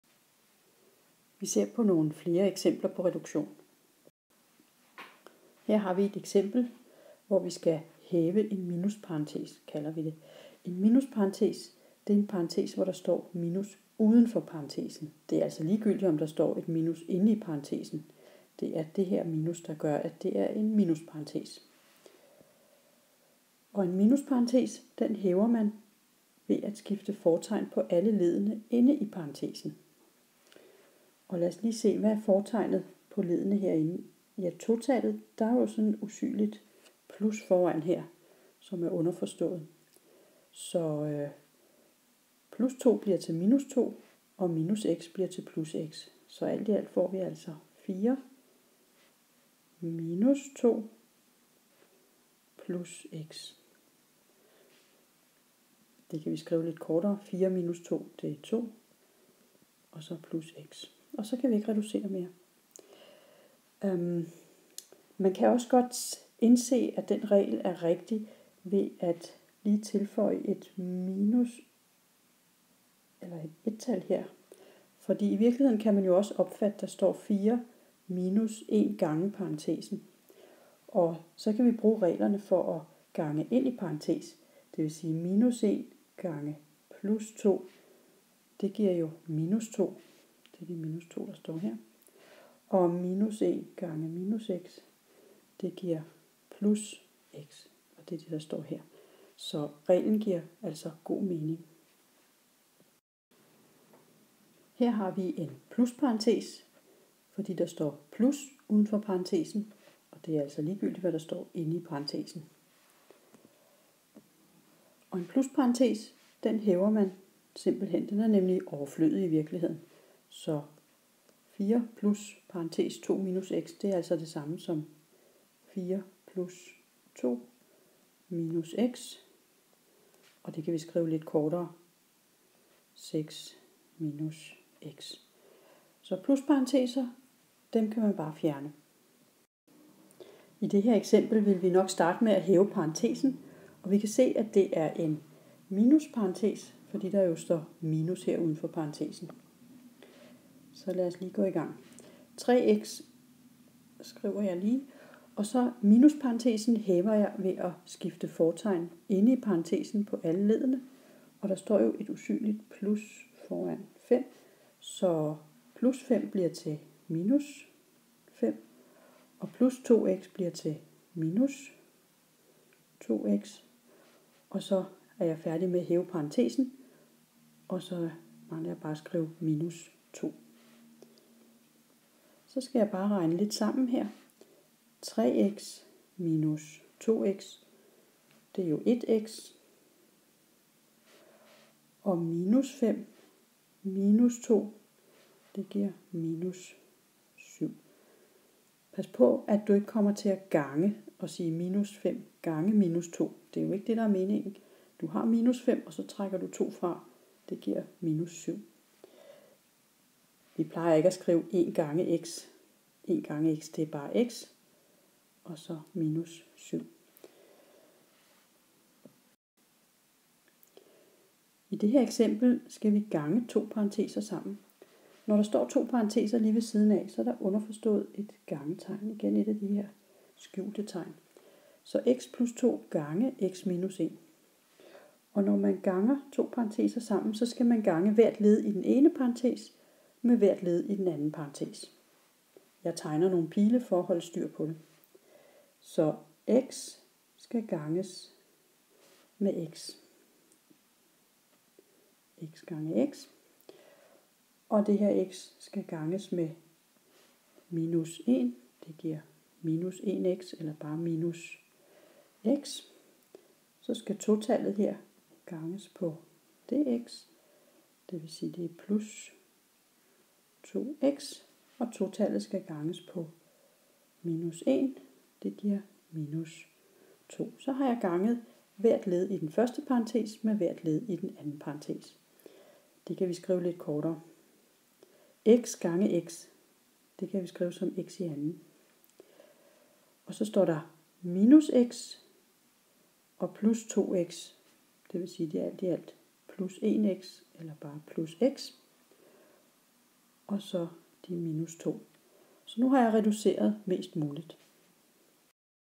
Reduktion 2
Flere eksempler på reduktion:
4 – (2 – x)
4 + (2 – x)
3x – (5 + 2x) – 2
(x+2)(x – 1)

Lavet med Explain Everything